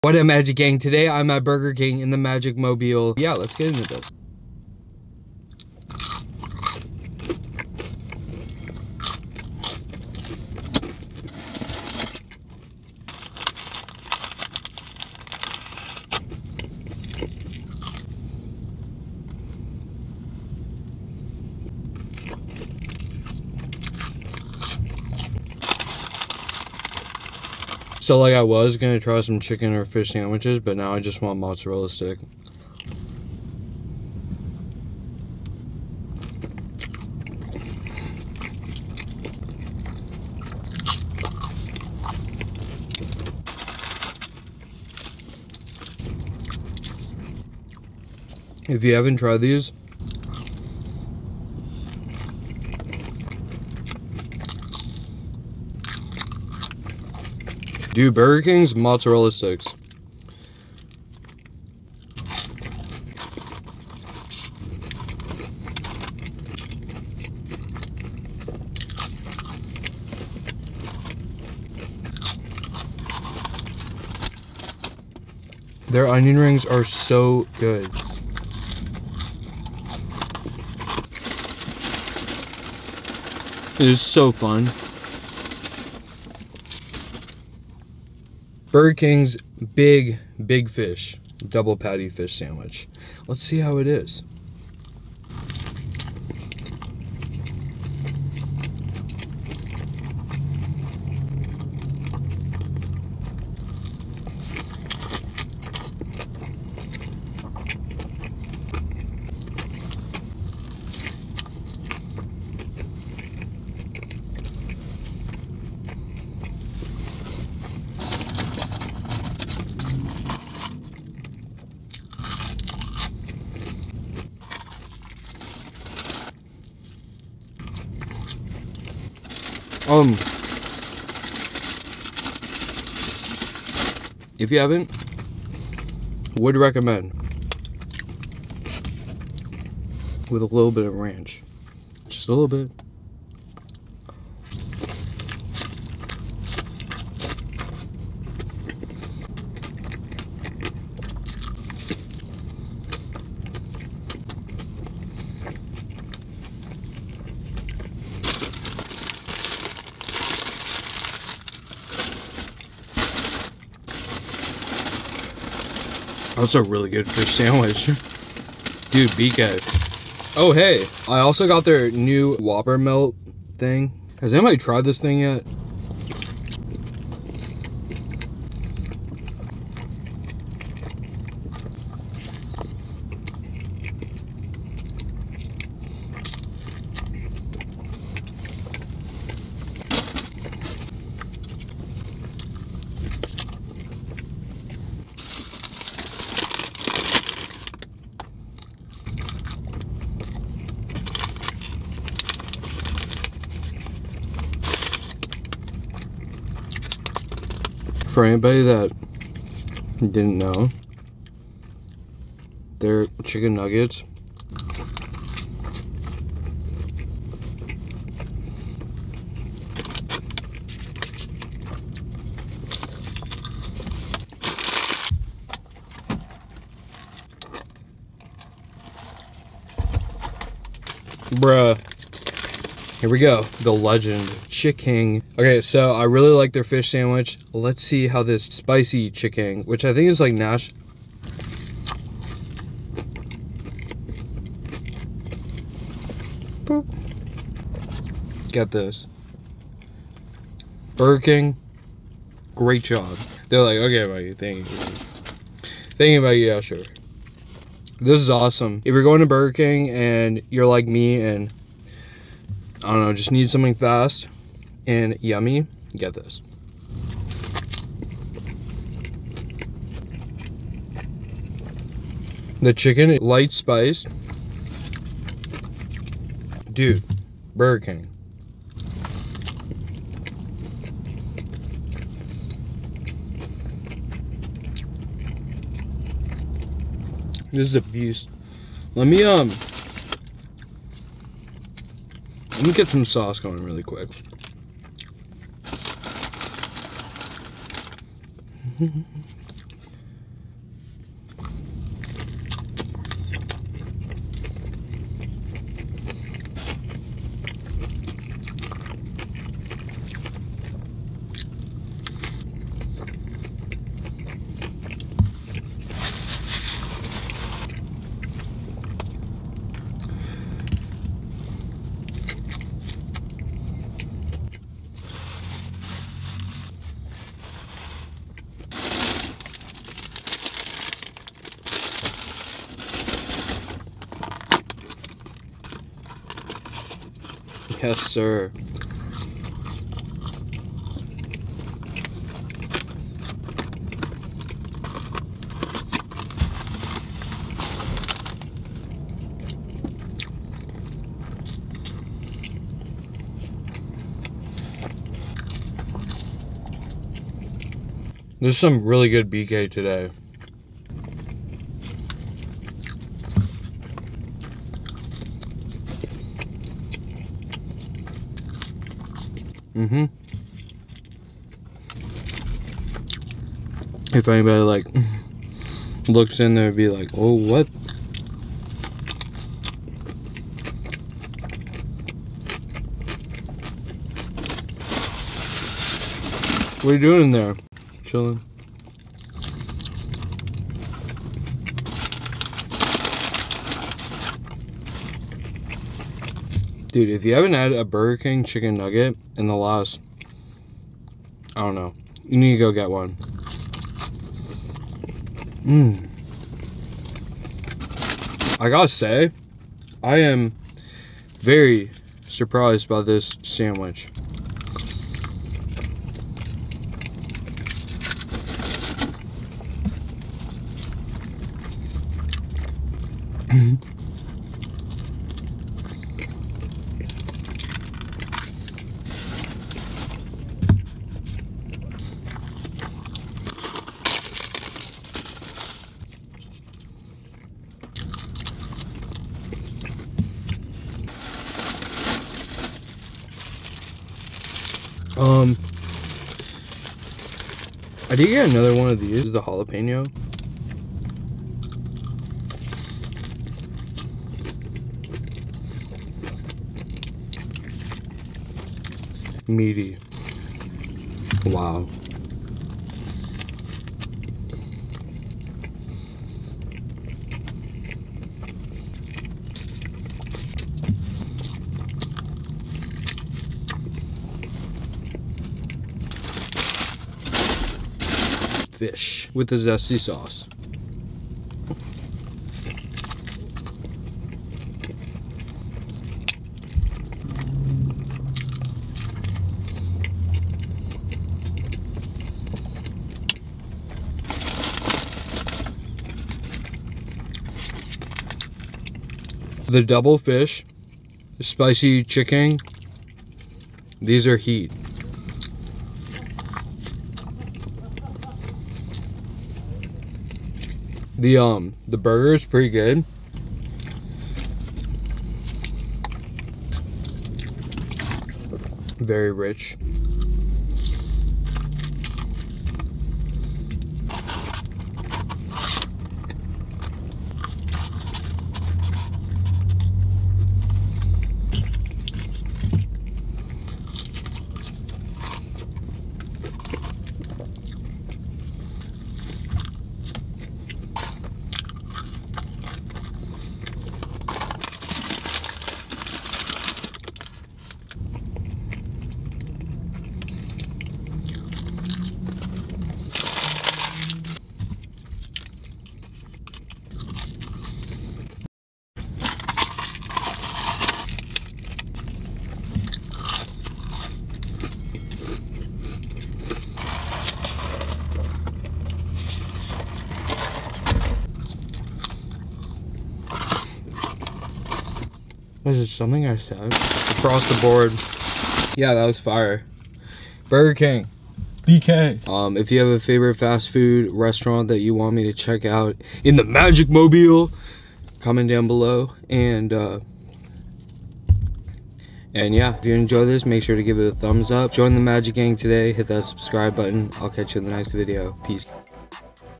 BIG BURGER KING MUKBANG WHOPPER MELT, HUGE Ch'KING, DOUBLE FISH Sandwich & More!
For today’s Car Mukbang I’m eating Burger King at Burger King! Burgers, Mozzarella sticks, BIG Fish Sandwich, Spicy Chicken sandwich, Whopper Melt, Chicken nuggets and more lets eat some BURGER KING!